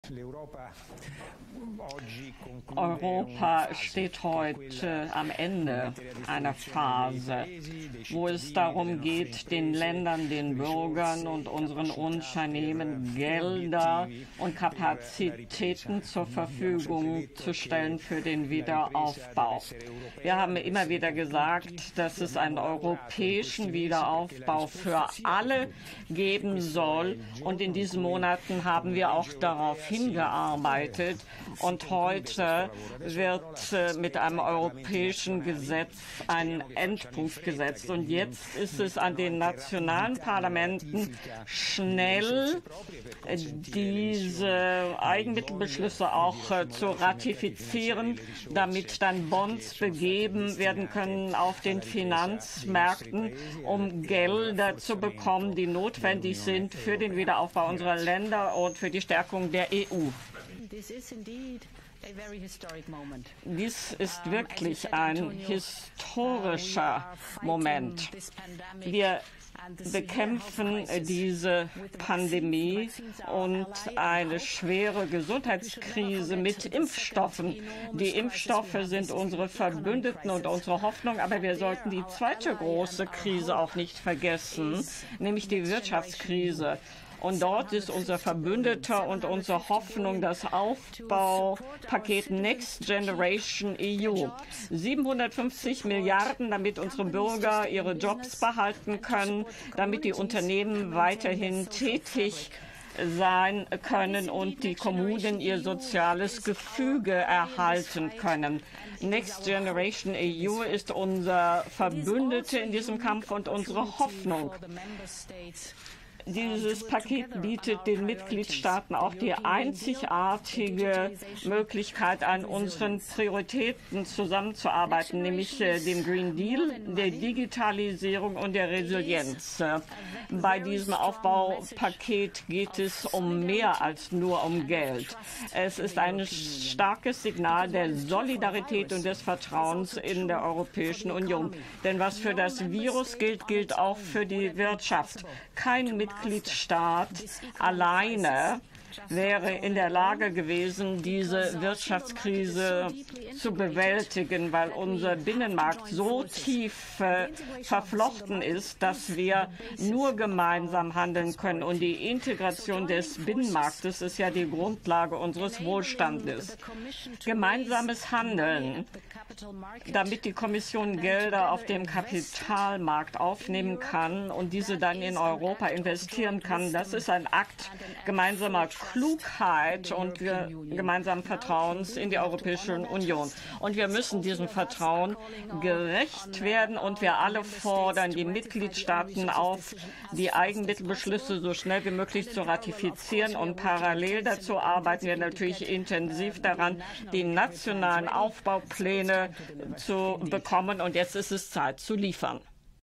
David Sassoli und Ursula von der Leyen zu EU-Corona-Hilfen
Pressekonferenz zur Unterzeichnung von EU-Corona-Hilfen mit u.a. David Sassoli (EU-Parlamentspräsident) und Ursula von der Leyen (EU-Kommissionspräsidentin)